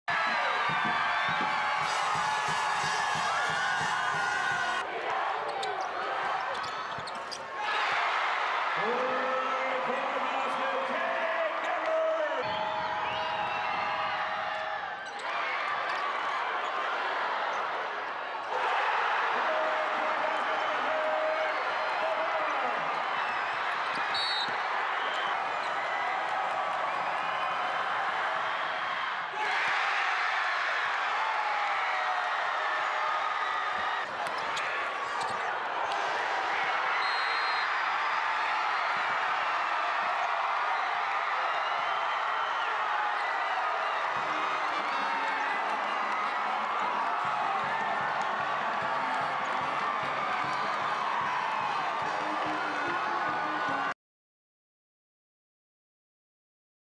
2B Boys Championship: Life Christian vs. Brewster
Brewster defeats Life Christian 58-57 to win the 2B boys championship title.